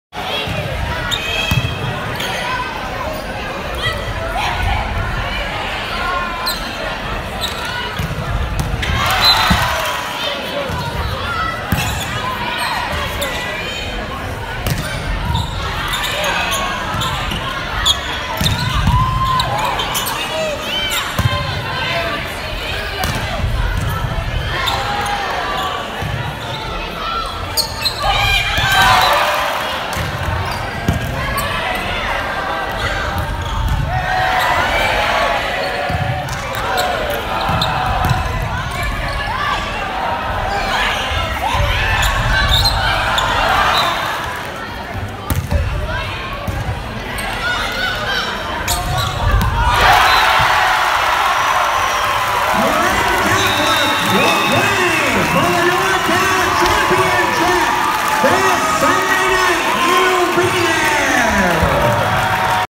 MC secures norcal final berth
MC defeats Turlock in norcal semis (Ian Ross/Marin Independent Journal)